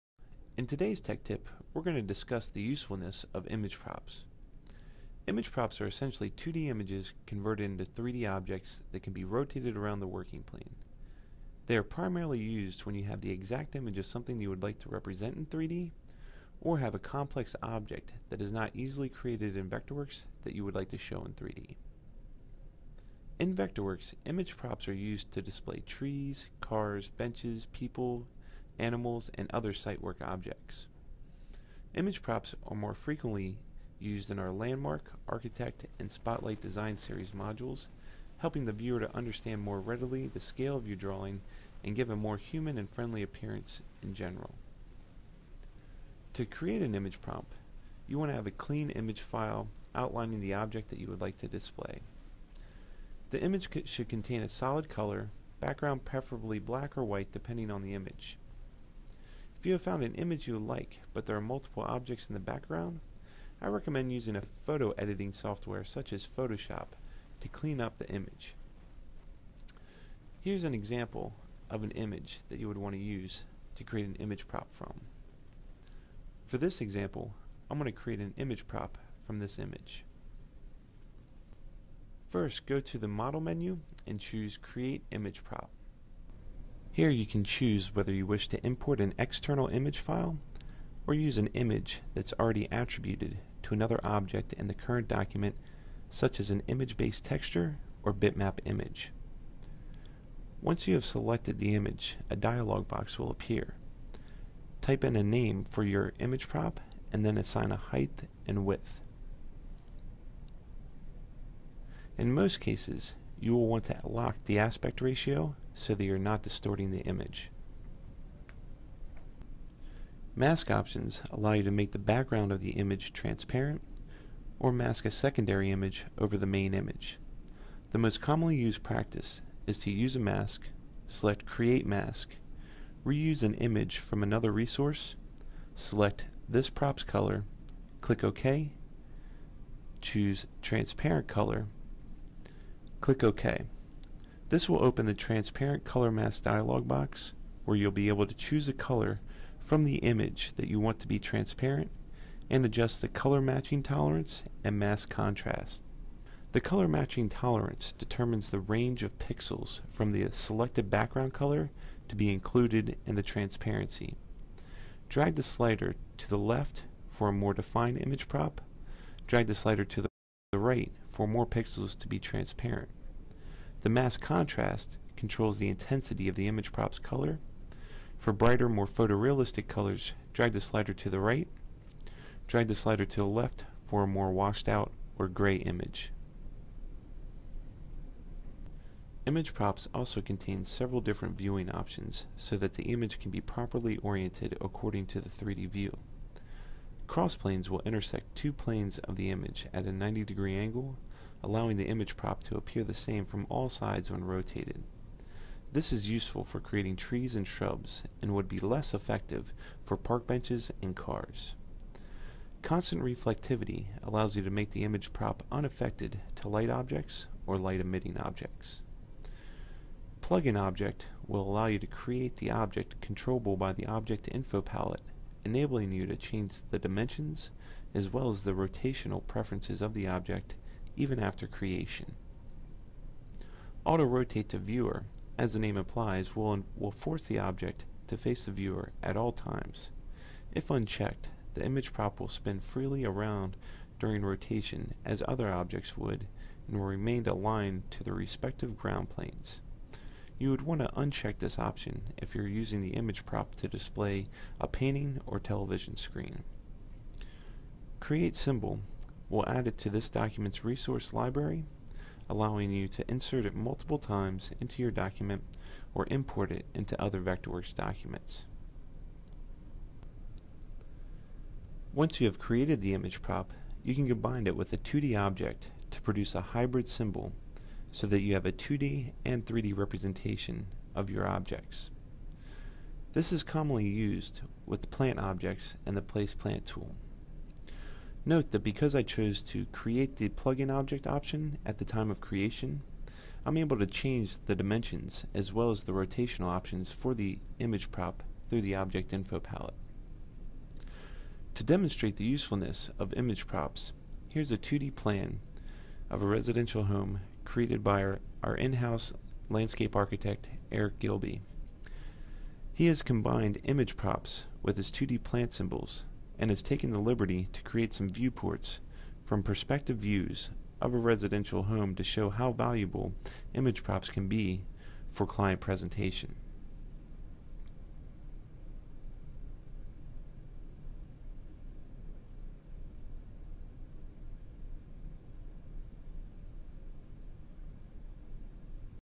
Creating Image Props in Vectorworks
Image Props: In this tech tip, we demonstrate step by step how to create image props